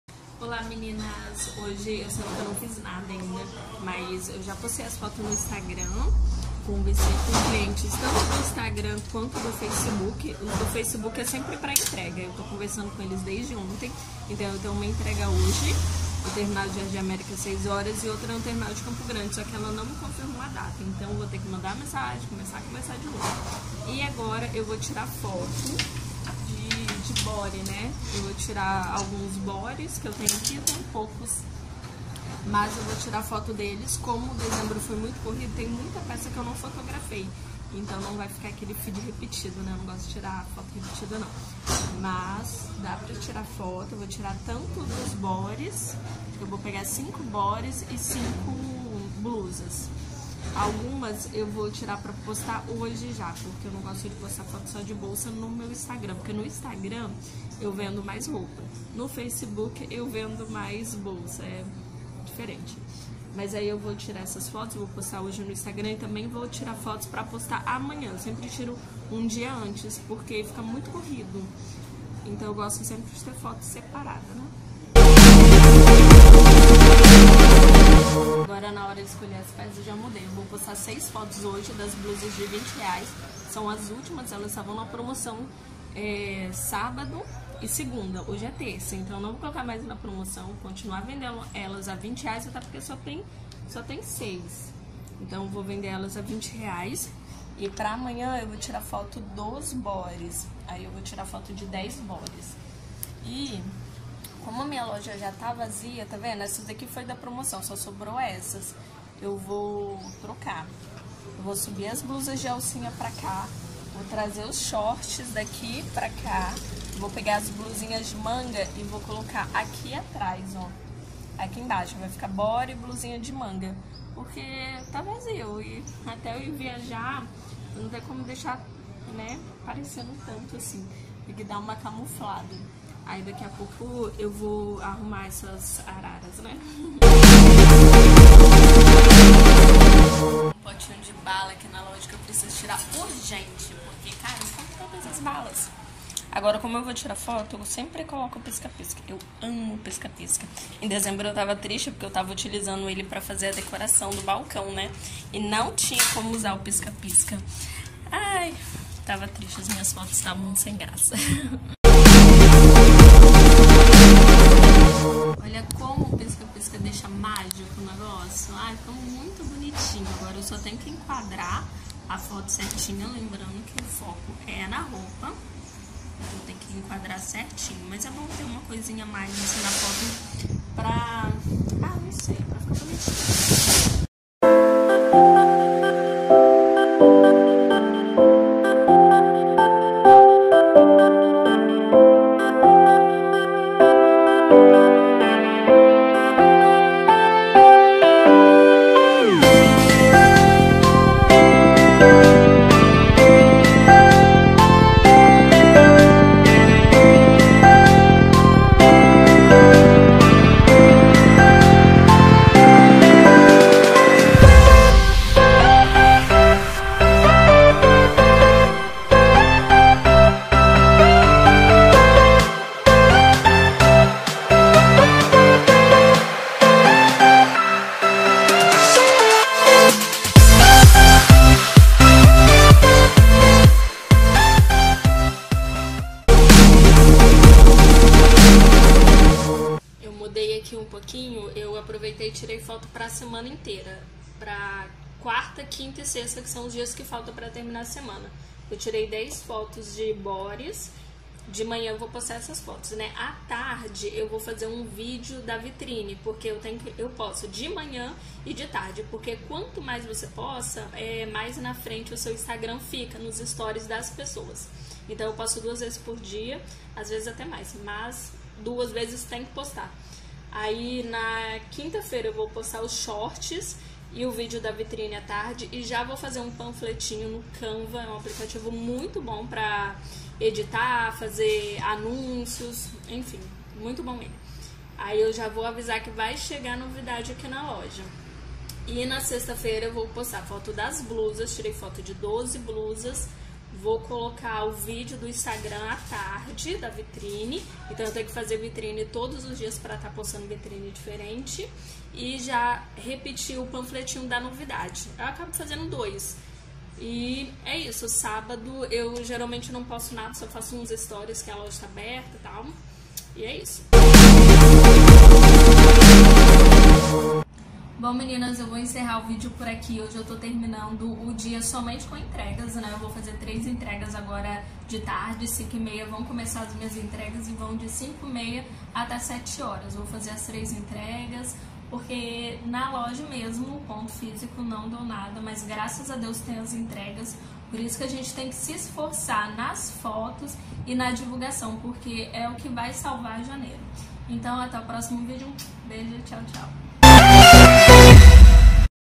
FOTOS DA SEMANA PARA O INSTAGRAM + ORGANIZAÇÃO DAS ARARAS.
Oi meninas, nesse vídeo falo um pouco de como são as minhas postagens para o instagram e arrumo as arraras para não parecerem tão vazias depois do natal e ano novo.

instagram loja @leonemoda